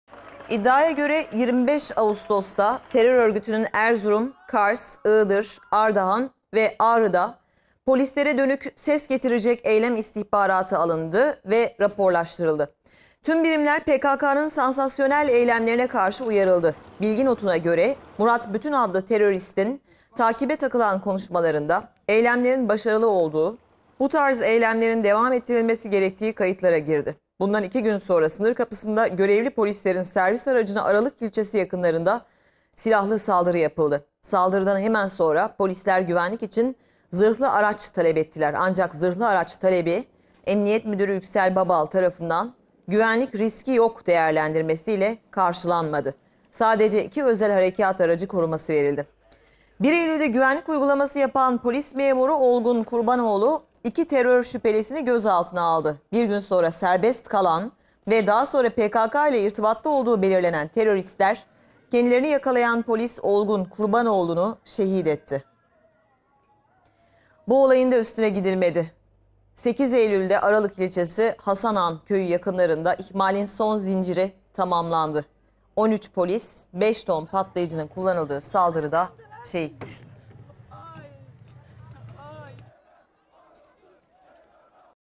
IĞDIR'DA 13 POLİS İHMALLER NEDENİYLE ŞEHİT OLDU İDDİASI
Iğdır'da 13 polis bile bile mi ölüme gönderildi? Zaman Gazetesi'nin haberine göre saldırıdan 13 gün önce istihbarat elde edildi. Hain tuzağı kuranlar dinlemeye takıldı. Ancak gerekli tedbirler alınmadı. İstihbarata rağmen zırhlı araç talebi Iğdır Emniyet Müdürü Yüksel Babal tarafından karşılanmadı.